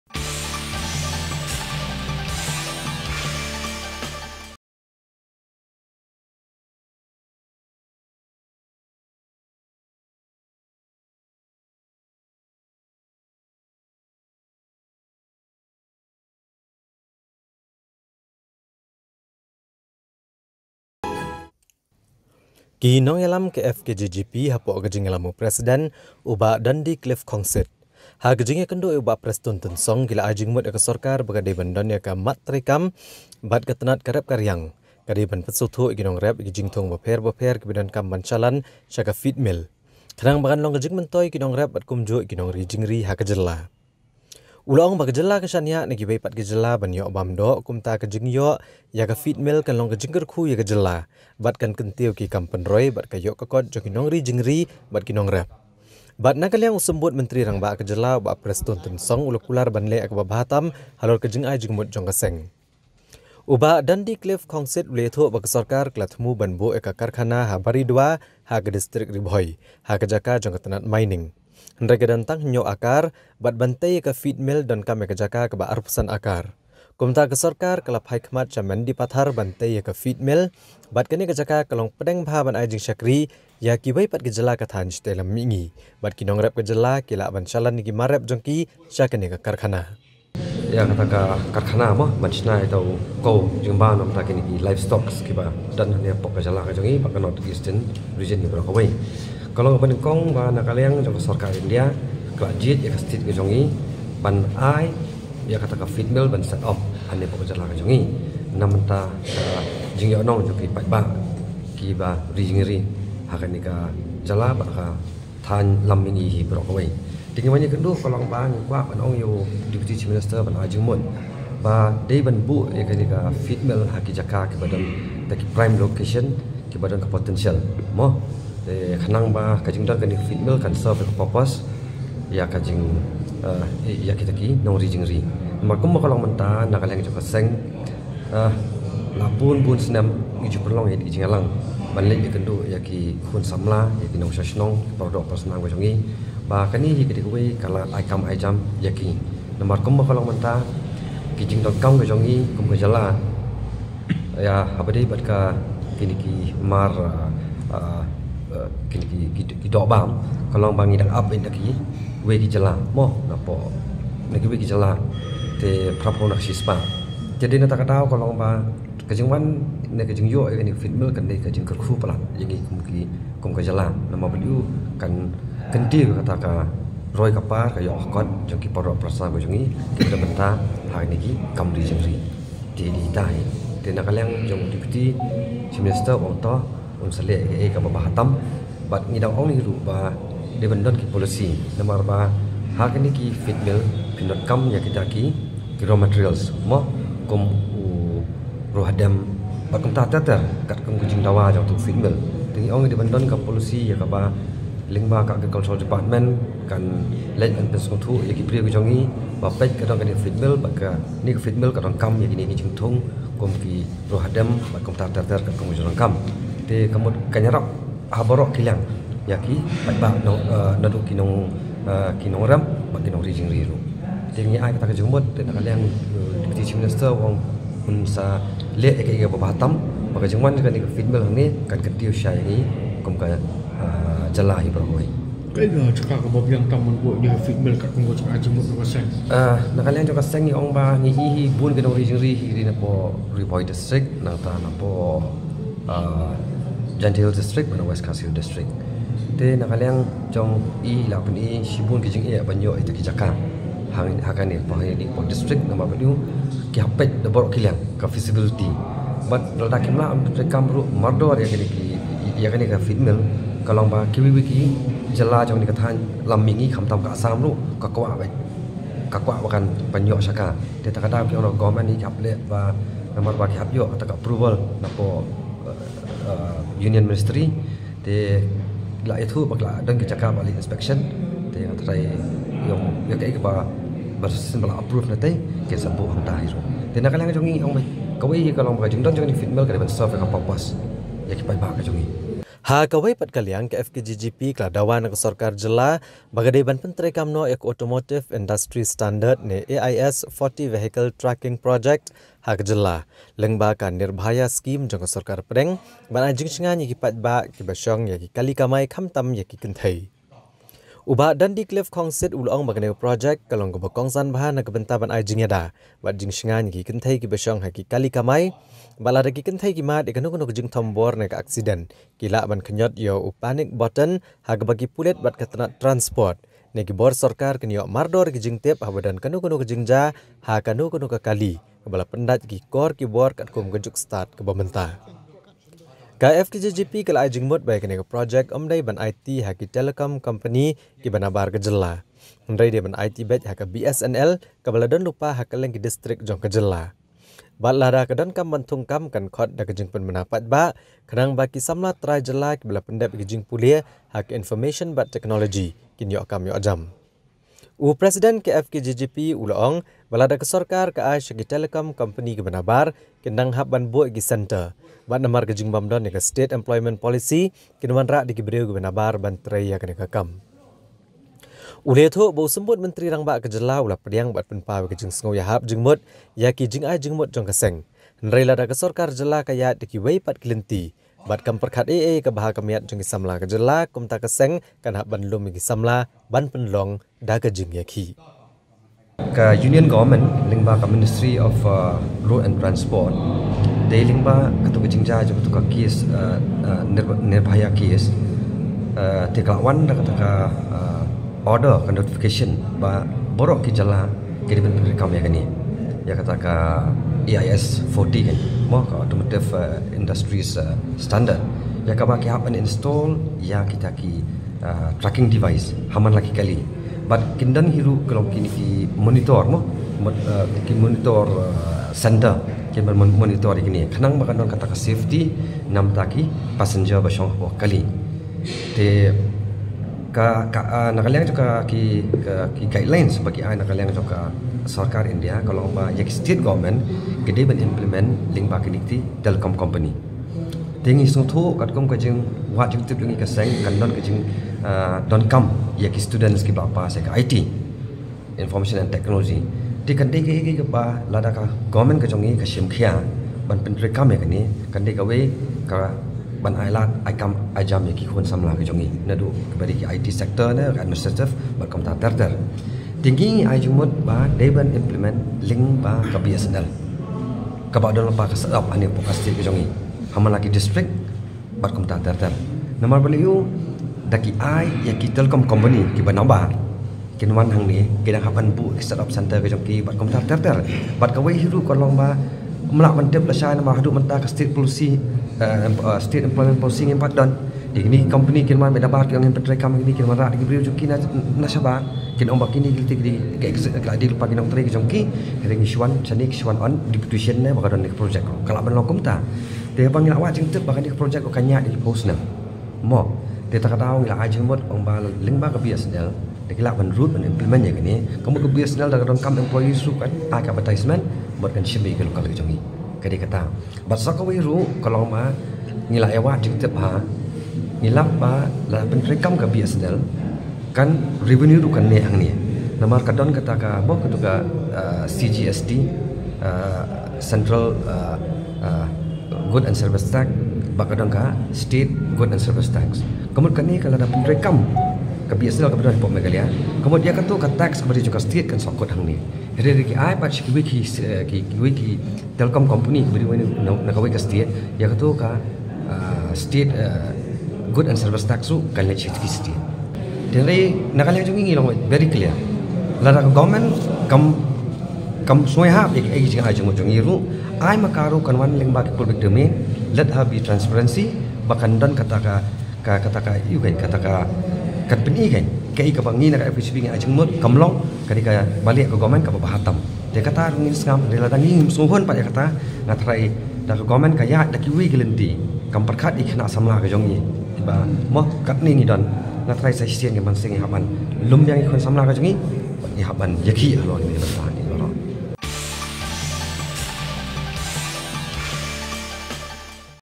Dawa Ka FKJGP Ban Buh Iaka Feed Mill Haka Jaka Kaban Ai Jingmyntoi Ia Ki Nongri Jingri & Nongrep
FKJGP Asks Govt  to Set Up Feed Mill for Farming Groups' Benefit